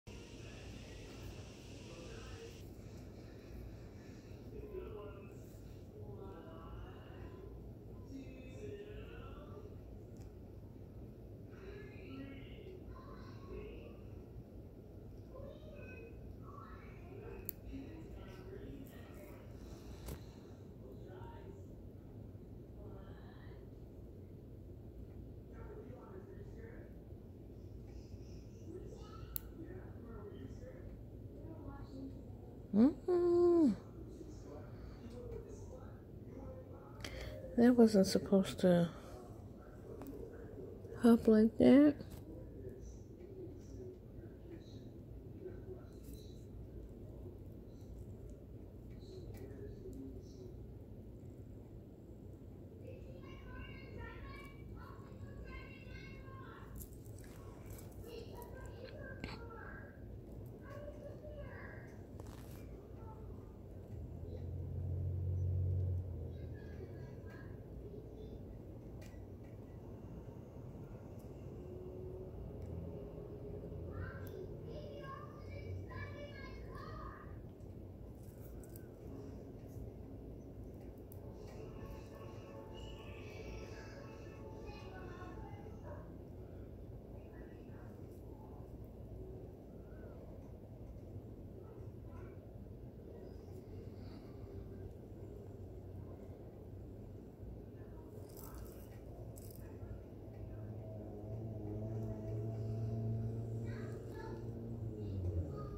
Juicy ingrown hair
Pulling ingrown hairs from a beard